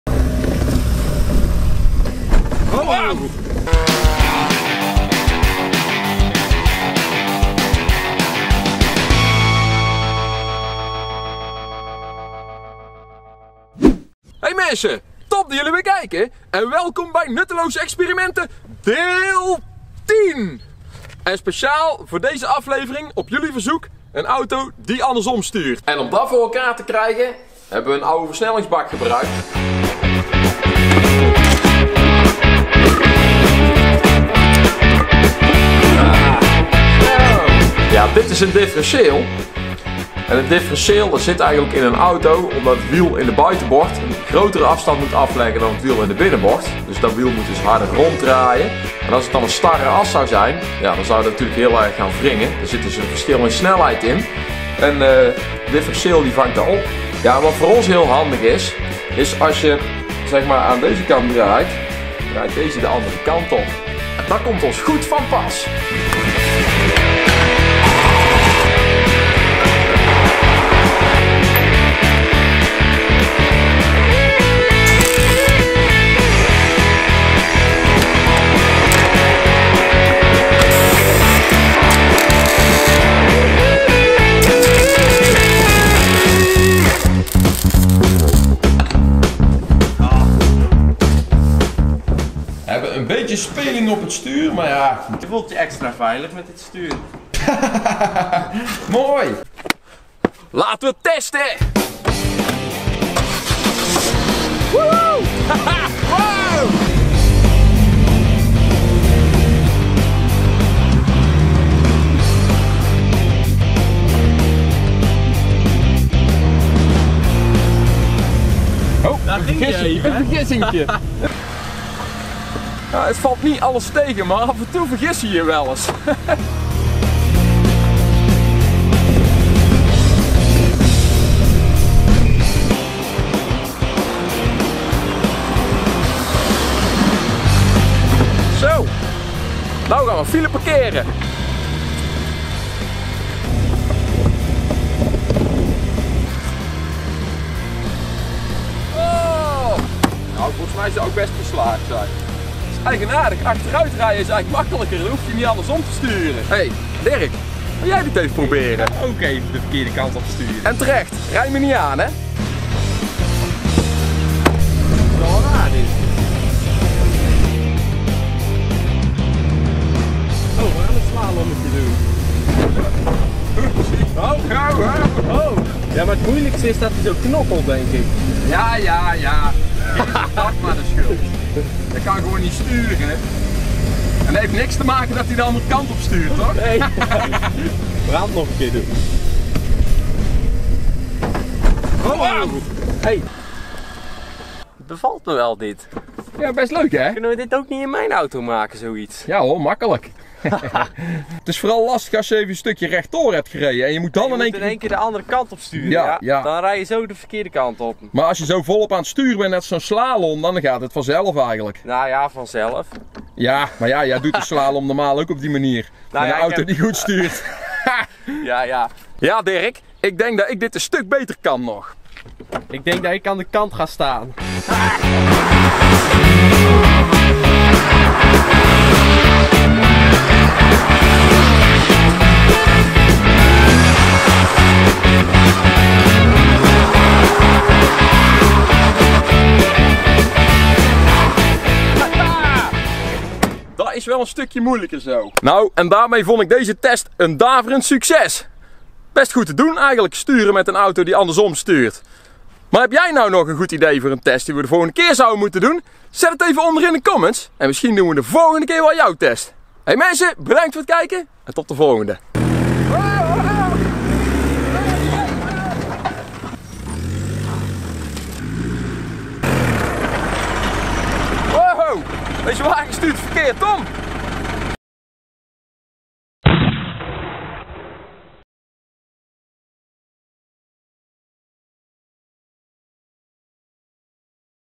Inversed steering car
On your request: a car with an inversed steering mechanism. How hard is it to drive? Post your request for next video in the comments!